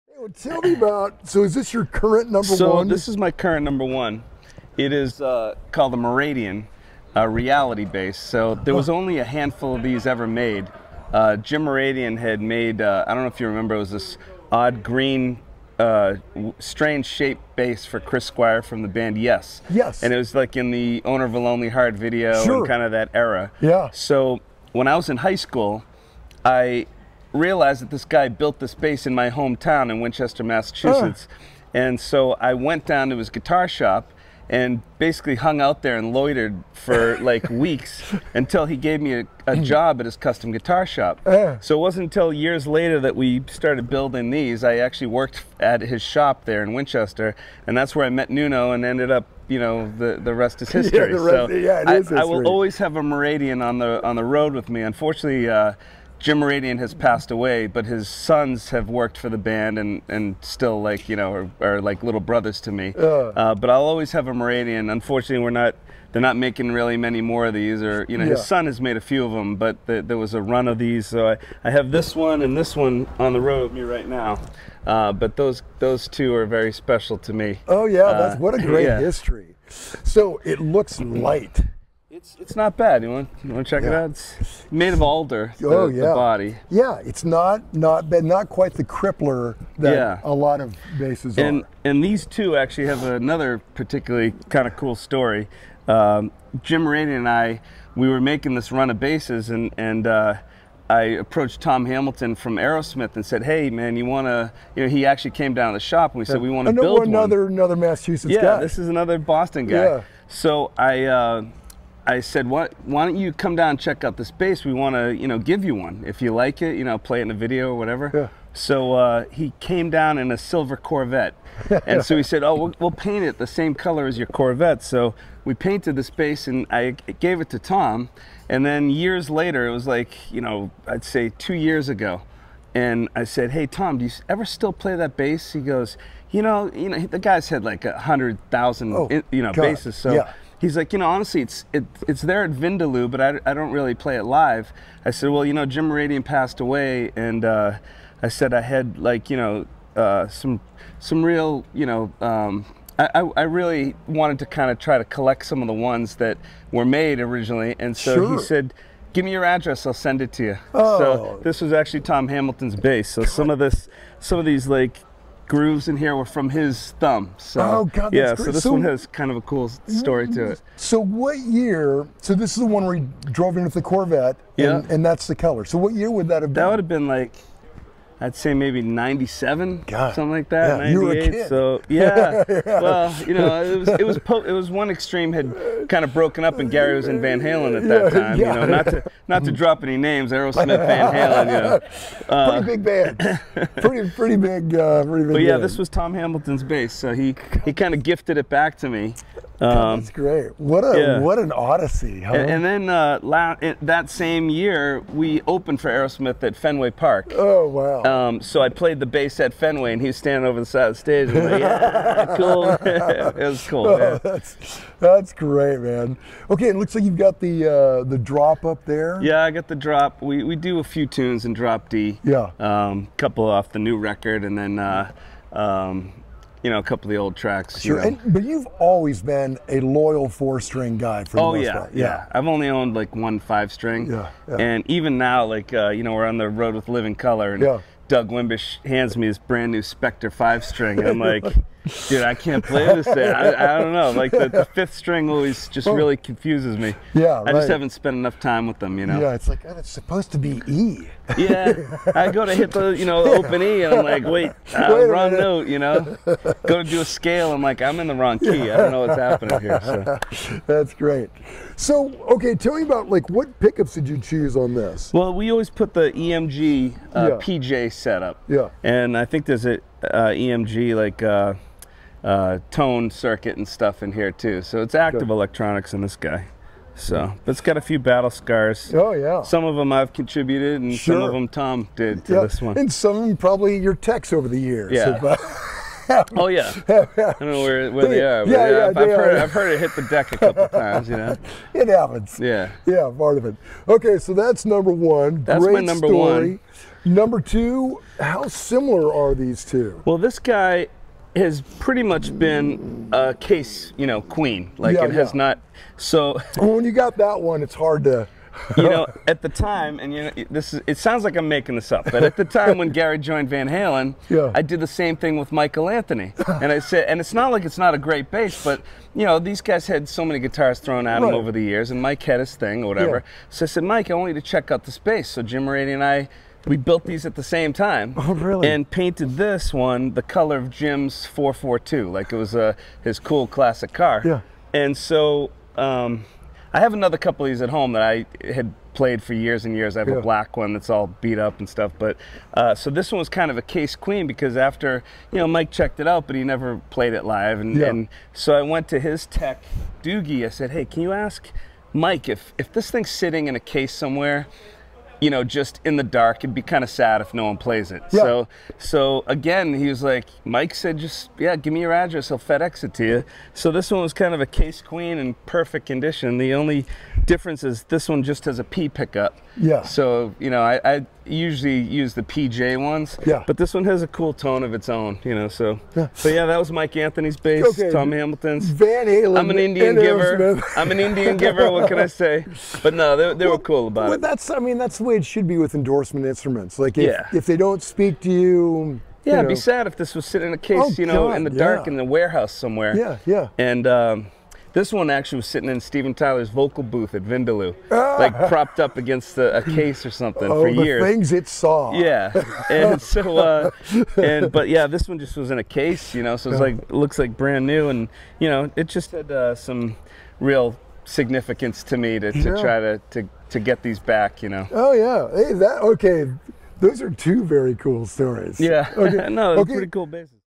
Aerosmith & Van Halen Used Pat Badger's Mouradian Basses Before Extreme
The sledgehammering bass player details a pair of bass guitars that were once owned by the low-end legends Tom Hamilton of Aerosmith and Michael Anthony of Van Halen.

Nearly 40 years ago, Nuno Bettencourt walked into Mouradian Guitars Co in Boston where Pat Badger was working and they formed a bond that would change their worlds. In March of 2024,  Pat Badger and Nuno Bettencourt,  of multi-platinum band Extreme, along with their tech, John Thayer, invited PG’s John Bohlinger to talk through their current rigs.

Rotosound UM45 UltraMag Type 52 Alloy Bass Guitar Strings - .045-.105 Standard 4-String